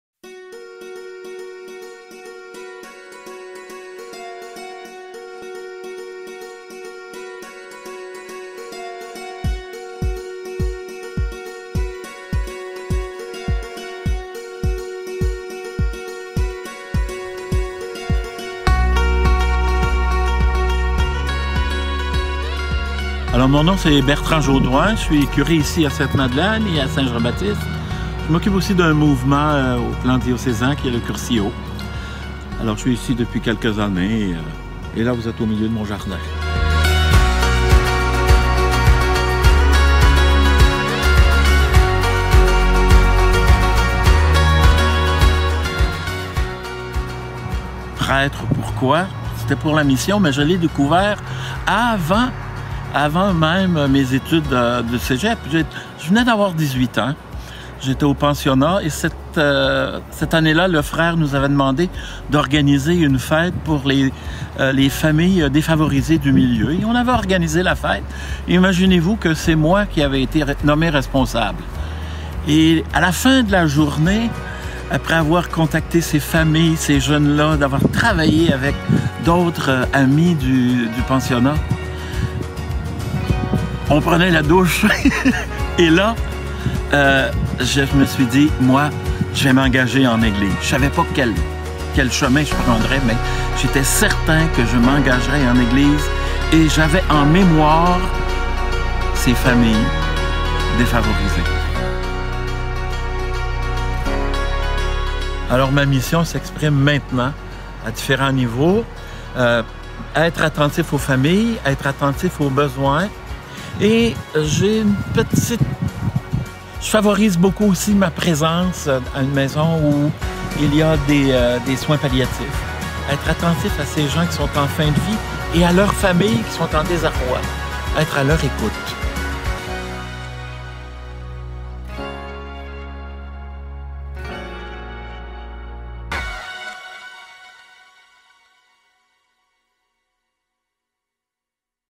BertrandJodoin moisdelamission20199
« Un jour, je m'engagerai en Église»: Bertrand Jodoin, prêtre au diocèse de St-Hyacinthe (Québec), nous raconte en quelques mots comment il vit aujourd'hui sa mission extraordinaire. Il a l'art de cultiver le bonheur !